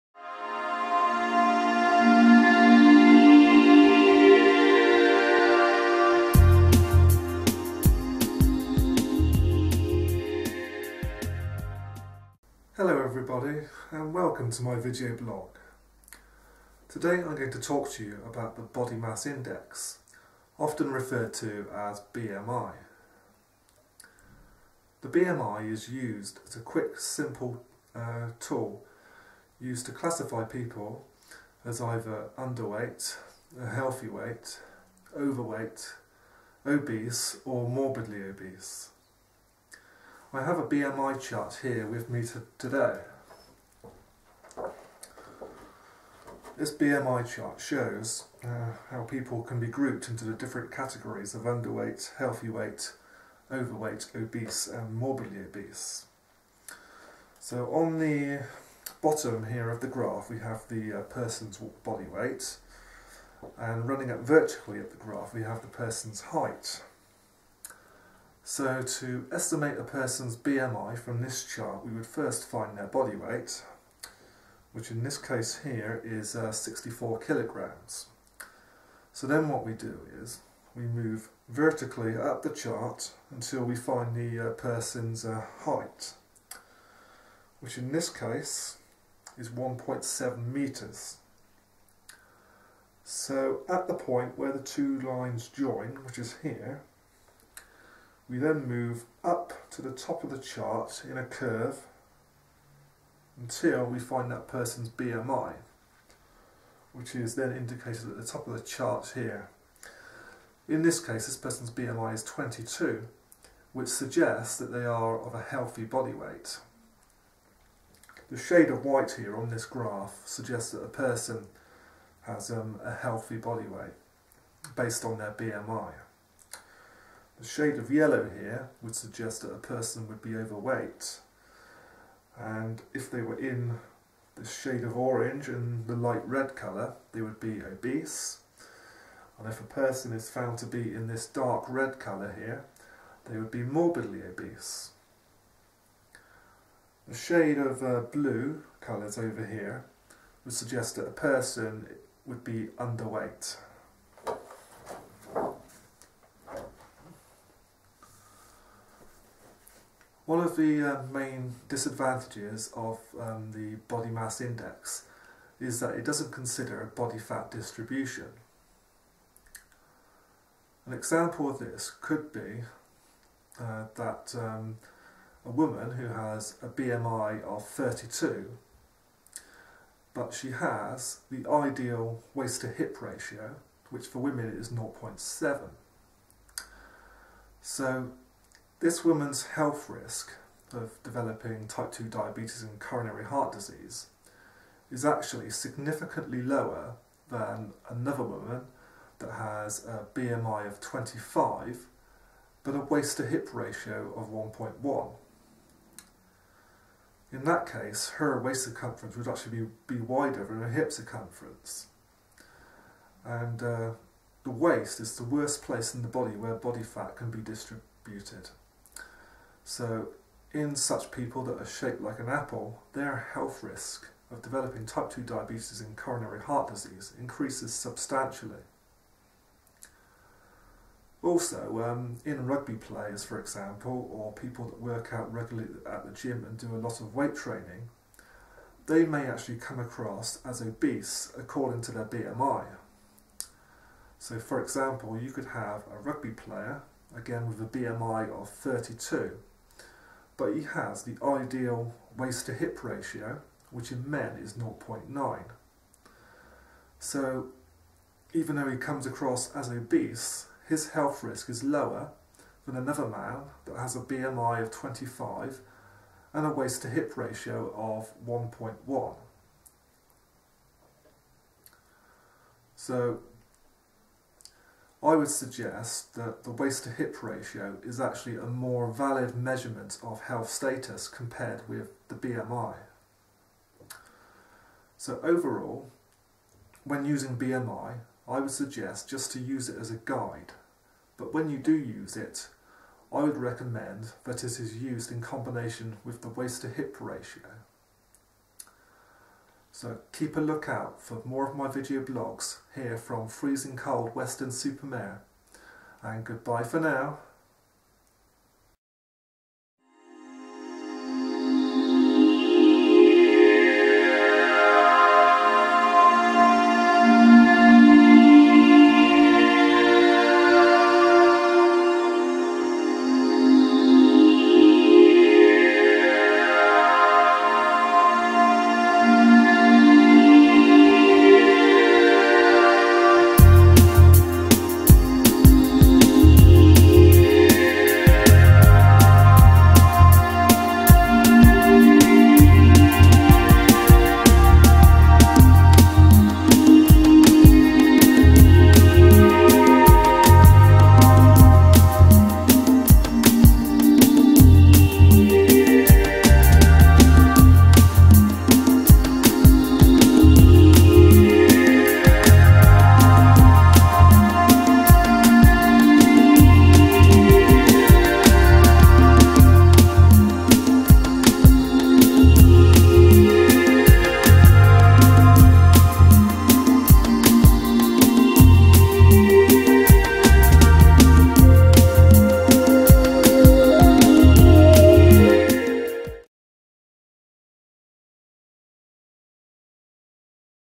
Body Mass Index Limitations
Nutritionist, Nutrition, Body Mass Index, Obesity, Overweight, Weight Loss, Weight Management, Waist to Hip Ratio, Coronary Heart Disease.

The Body Mass Index or the BMI is a quick and simple calculation for classifying people as underweight, healthy weight, overweight, obese or morbidly obese.  However, BMI does not consider body fat distribution.  I would argue that the waist to hip ratio is a more valid measurement of health status.  A woman who is obese with a BMI of 32, but has an optimal waist to hip ratio of 0.70 has a lower health risk than another woman with a BMI of 25 and a waist to hip ratio of 1.1.  When using BMI, it is best to use it in combination with waist to hip ratio.